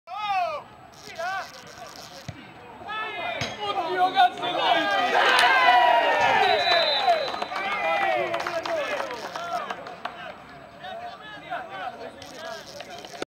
Campionato dilettantistico.Di seconda categoria girone D (VT)
Sport dilettantistico.Oggi alle ore 15,00 presso il campo sportivo di Capena (Roma), si e svolta la partita di calcio , di seconda categoria Lazio girone "D" (VT) , tra il Civitella e Corchiano. Risultato finale Civitella 3. Corchiano 1. Buona domenica a tutti.